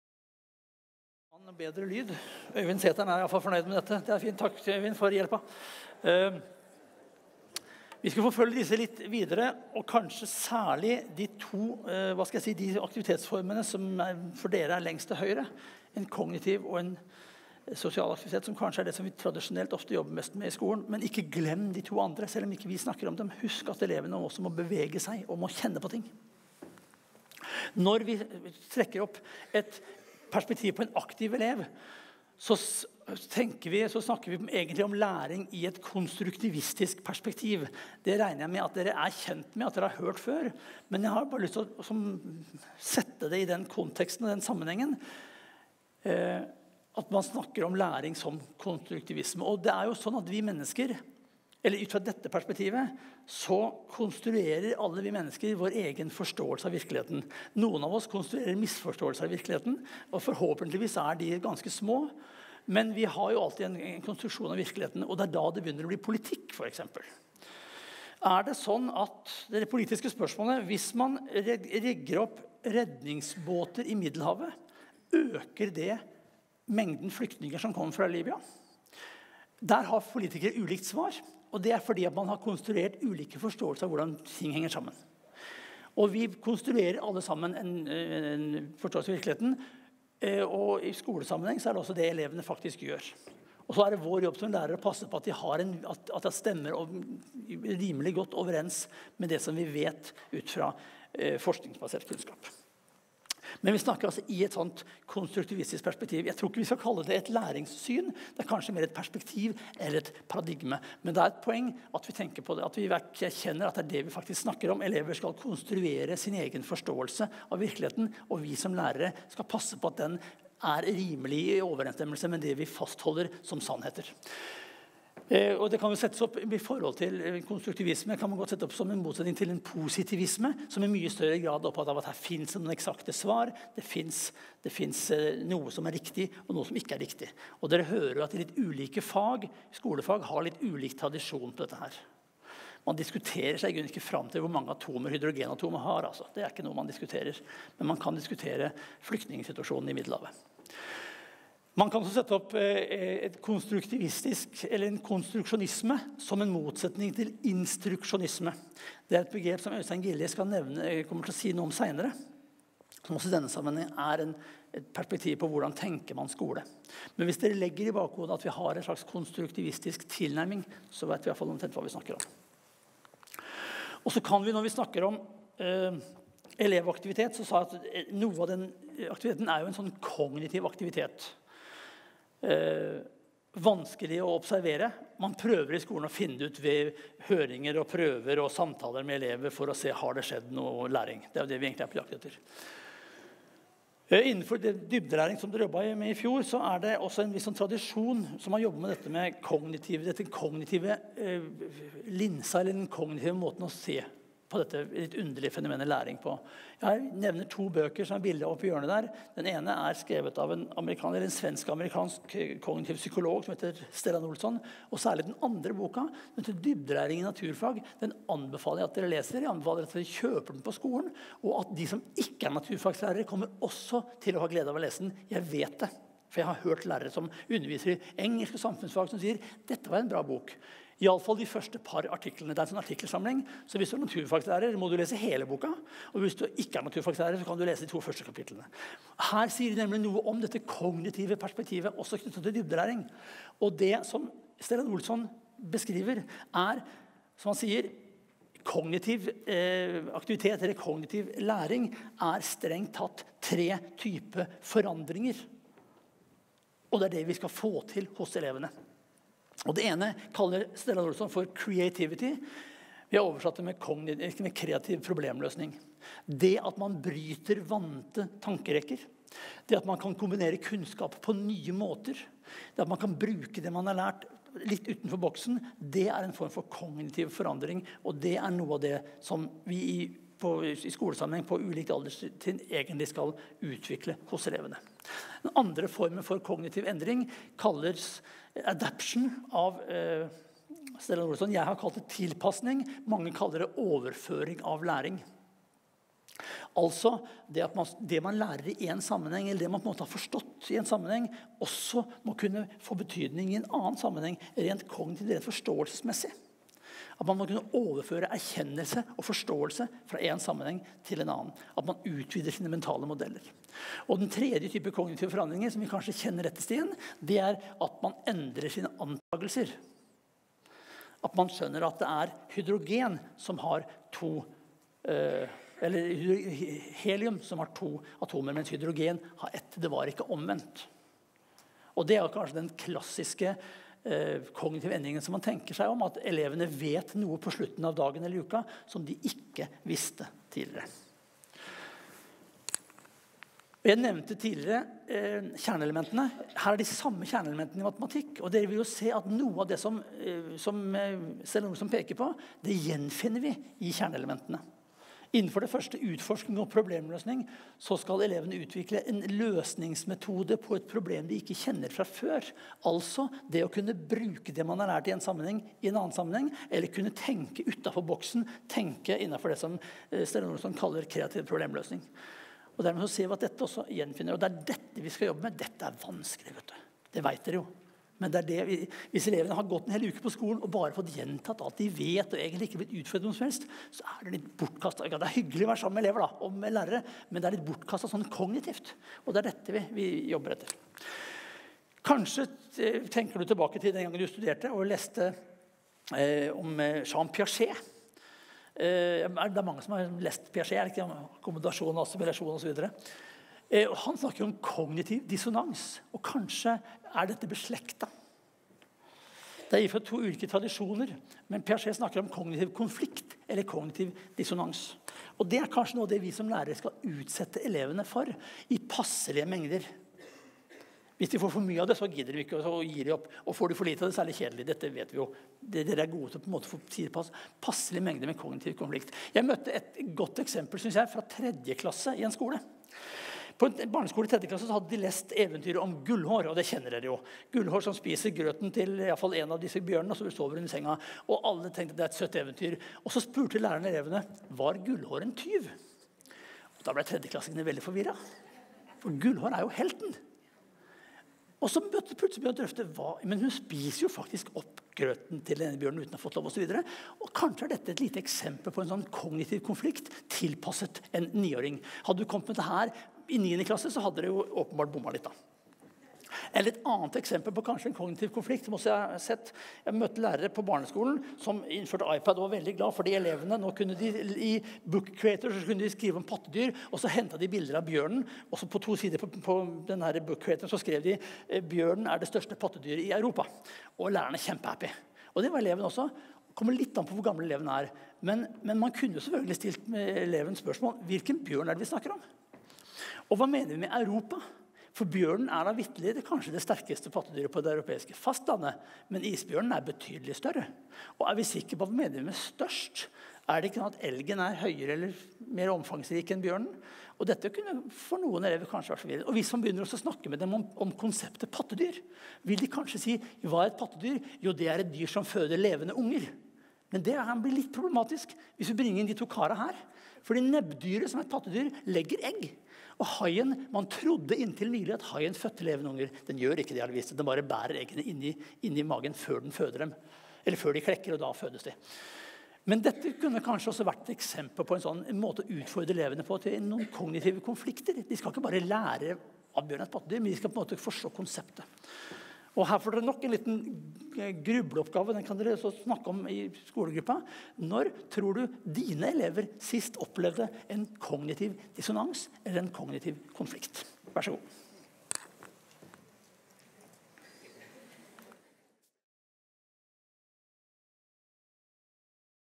Del 3 av 4: Elevrollen i fagfornyelsen ved Bjørn Bolstad
I denne videoserien bestående av fire deler foreleser Bjørn Bolstad, seniorrådgiver i FIKS, om elevrollen i fagfornyelsen. Opptaket er fra en workshop på Radisson Blu Alna 5.-7. november 2019.

Videoen kan brukes fritt til deling og opplæringsformål, så lenge enheten FIKS (Forskning, innovasjon og kompetanseutvikling i skolen) ved Universitetet i Oslo er kreditert.